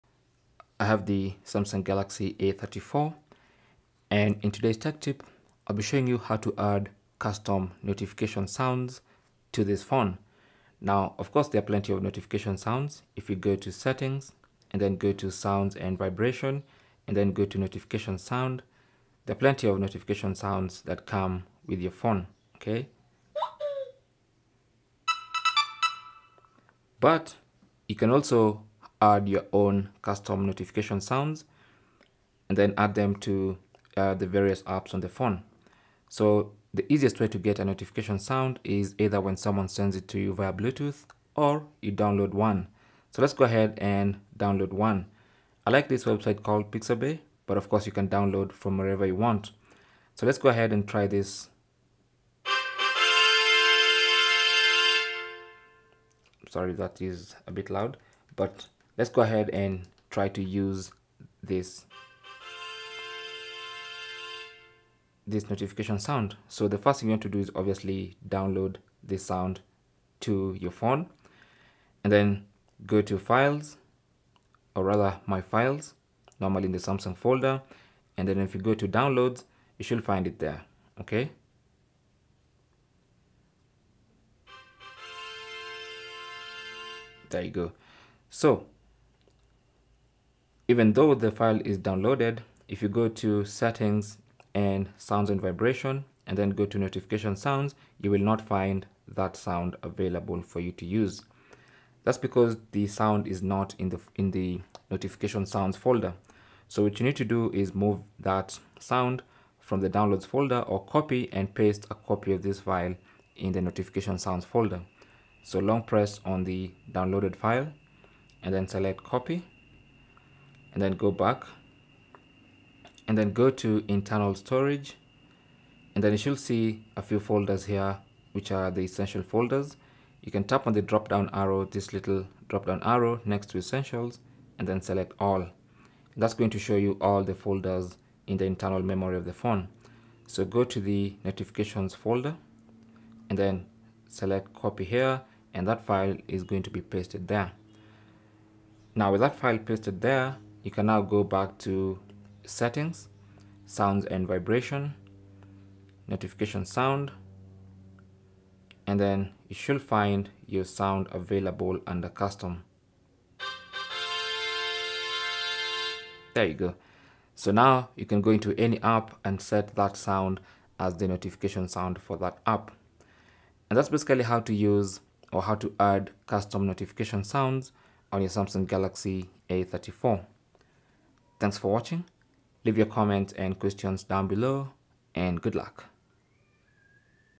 How To Add Custom Notification Sounds On Samsung Galaxy A34 5G
Here is a step-by-step guide on how to add custom notification sounds on Samsung Galaxy A34 5G. While the device comes with a range of preloaded notification sounds, adding custom notification sounds can add a personal touch to your device.

The Samsung Galaxy A34 5G is a powerful device that offers an immersive experience for users. One of the features that add to this experience is the ability to customize notification sounds.

Add Custom Notification Sounds On Samsung Galaxy A34 5G
Step 1: Download Notification Sound

The first step to adding a custom notification sound is to download the sound you want to use. You can find a range of notification sounds online, including music tracks, sounds effects, and even voice recordings. Ensure that the sound is in a compatible format, such as MP3 or WAV.

Step 2: Transfer Sound To Your Device

Once you have downloaded the sound, transfer it to your Samsung Galaxy A34 5G. You can do this by connecting your device to your computer using a USB cable and copying the file to the device's internal storage or SD card. Alternatively, you can download the sound directly to your device using a browser or email attachment.
You can also have it sent to you via file transfer or via Bluetooth.

Step 3: Locate The Sound File

After transferring the sound file to your device, locate it using the "My Files" app. The app allows you to browse through the files on your device, including music, documents, and downloads. Find the folder where you saved the sound file and select it.
If you downloaded it, it should be in the "Downloads" folder.

Step 4: Move Downloaded Custom Sound

Once you have located the sound file, tap and hold it until a menu appears. Select the "Copy" option from the menu. Go back to the My Files menu and select "Internal Storage".

Tap on the drop-down arrow near 'Essentials' and select "All". This will reveal all the folders in the internal storage.

Find and choose the "Notifications" folder. Copy the downloaded file there.

Step 5: Set As Notification Sound

Now you can go to the Settings app, tap on "Sounds and Vibration". Select "Notification sound" and you should find the downloaded custom notification sound under "Custom".

You can also use this option to set the sound as your ringtone, alarm, or contact tone.

Cell Phone Tripod Adapter